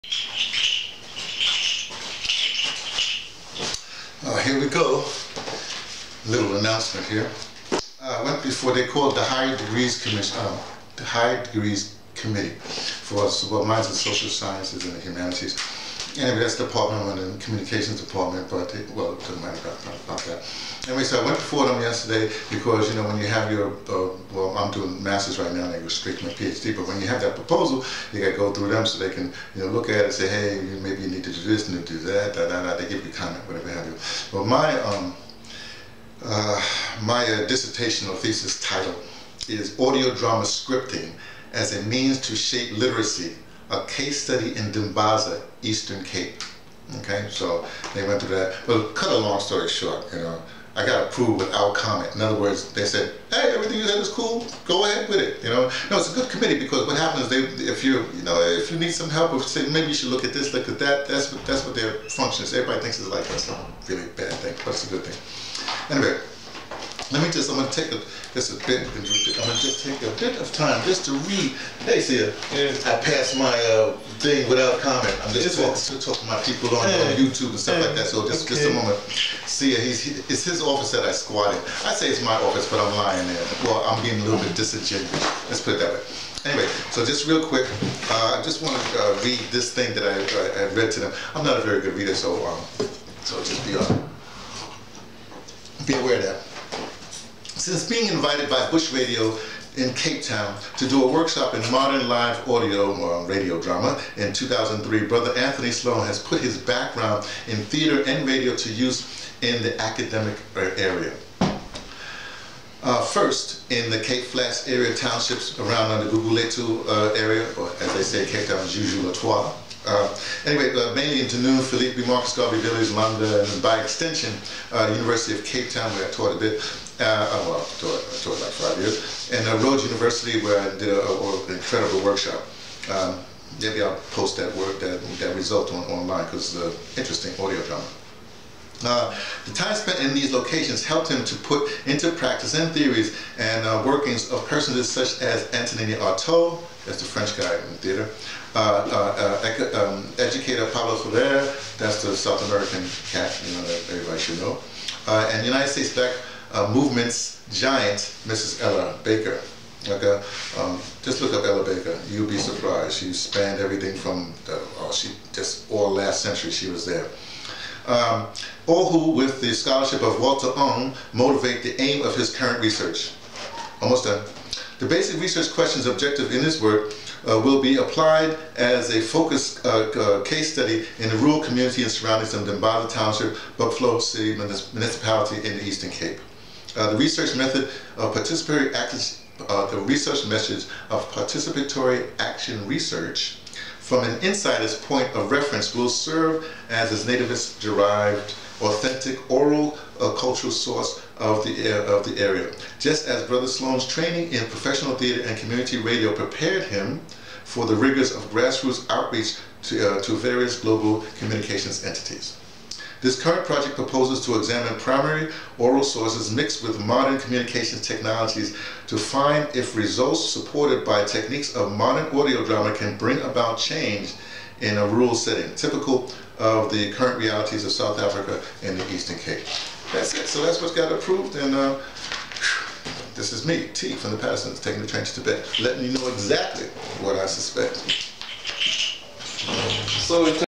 Approval of my dissertation / thesis
My dissertation / thesis titled Audio Drama Scripting as a means to shape literacy: A case study in Dimbaza, Eastern Cape.
Now the work continues toward graduation next year.
I respect and even enjoyed the process.
And i really love my topic.

Know that i will be going straight from the Masters to the Ph.D as that was the original intent and is necessary as it really is a Community Development project that will take between 3 and 5 years.
I'm very excited.

BITCOIN CONTRIBUTIONS:
722fce22-5e9f-42fc-a378-2da90f5d267a